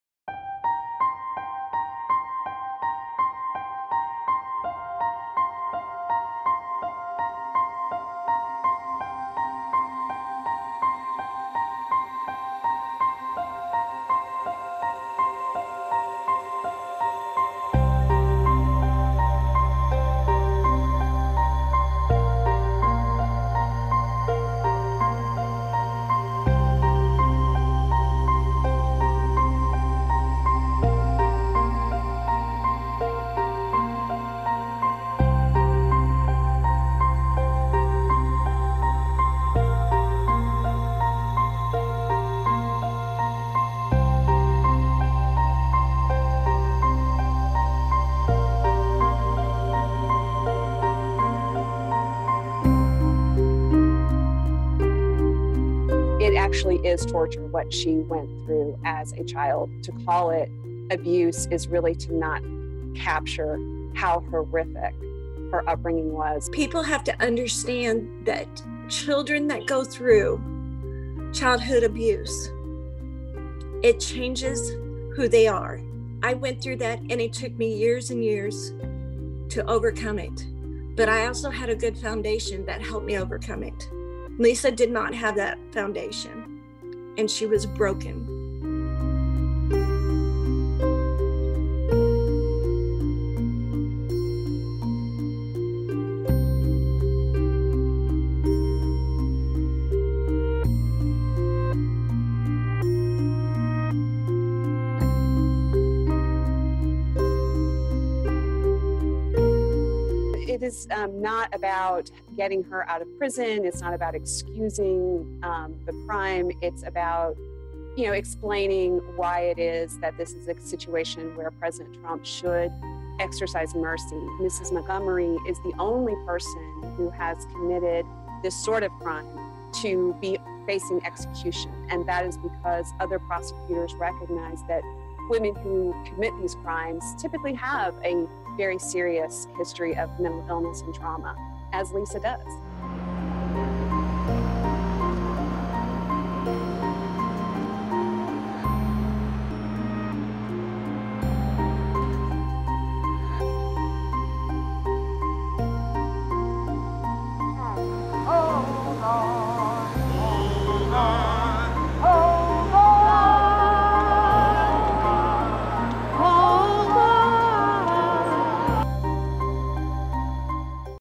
Lisa Montgomery, the first woman to be executed by the US government in nearly 70 years | DW News
Lisa Montgomery could be the first woman to face the death penalty in nearly 70 years. She was convicted for murdering a pregnant woman Bobbie Jo Stinnett and taking her unborn child, trying to pass the baby girl off as her own. In spite of her gruesome crime her supporters say she doesn’t deserve to die. As a child Montgomery was a victim of severe abuse. Her lawyers say she suffered
incest, gang-rape and brain damage.
The 52-year-old is being held in a prison in Indiana. She’s scheduled to die by lethal injection on January 12th. Multiple courts have rejected her appeals. Only outgoing President Donald Trump could stop the execution.
Under Trump, the US restarted federal executions for the first time in almost two decades. Montgomery is one of three inmates scheduled to die before his term ends. Trump’s successor Joe Biden once supported the death penalty but has now pledged to end its use.